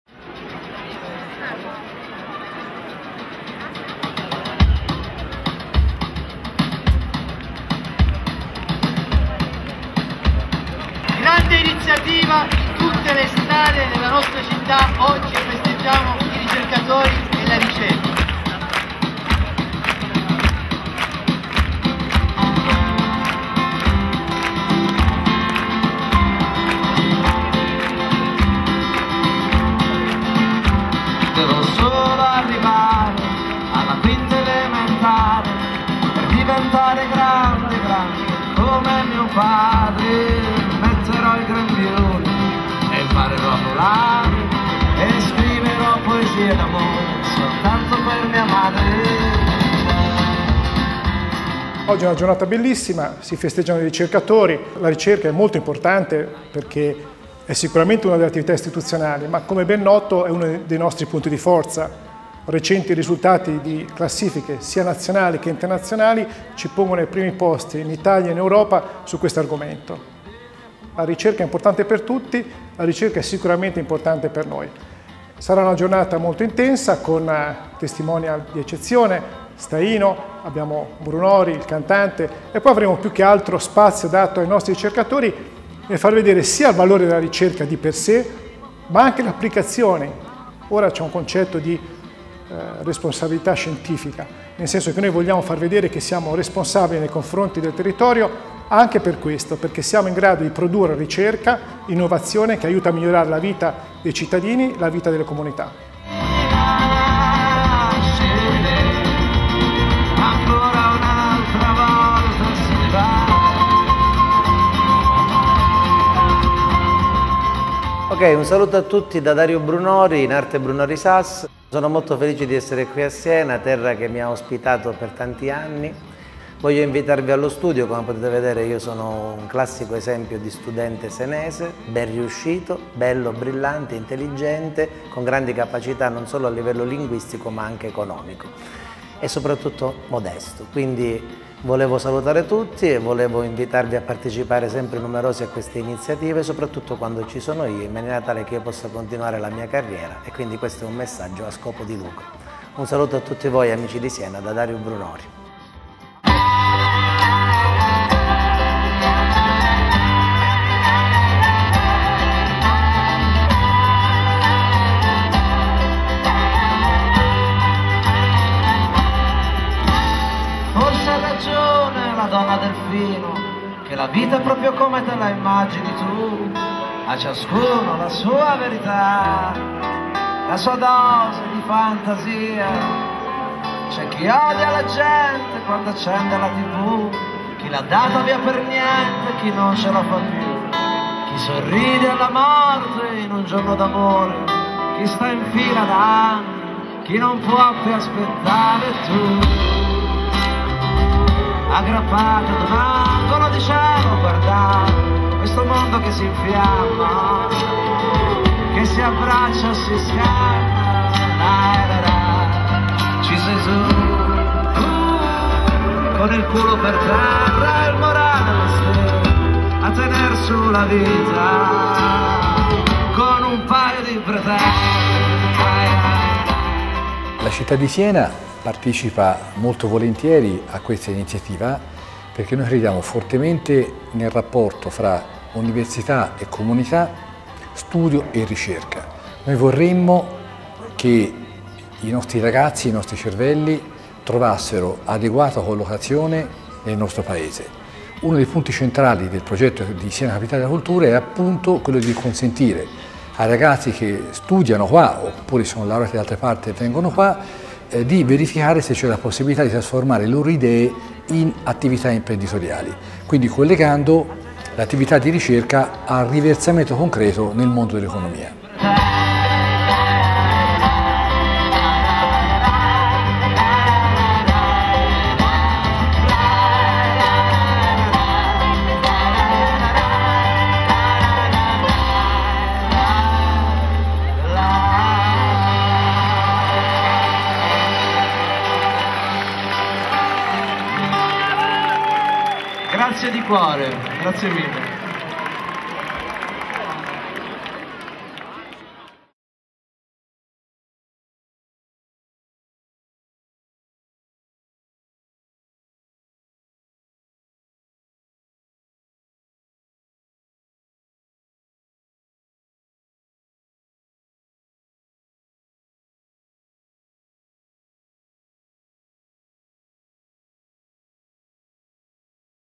Bright 2014 - La notte dei ricercatori in Toscana
Il 26 settembre si è tenuta l'edizione 2014 di Bright! La notte dei ricercatori in Toscana.
A Siena l'evento, inaugurato da Enrico Rossi presidente della Regione Toscana, ha visto la nutrita partecipazione dei ricercatori dell'Università di Siena che hanno animato palazzi, piazze e vie della città con esibizioni, esperimenti, seminari e spettacoli.

Guest Star dell'edizione 2014 il cartoonist Sergio Staino e il cantante Dario Brunori che hanno dedicato delle performance ad hoc alla ricerca e alla Notte dei ricercatori.

Bright - La notte dei ricercatori in Toscana - Siena
Promosso dalla Regione Toscana
Organizzato da Università di Siena, Comune di Siena, Banca Monte dei Paschi di Siena, Fondazione Toscana Life Sciences, Siena 2019, Città candidata a Capitale Europea della Cultura.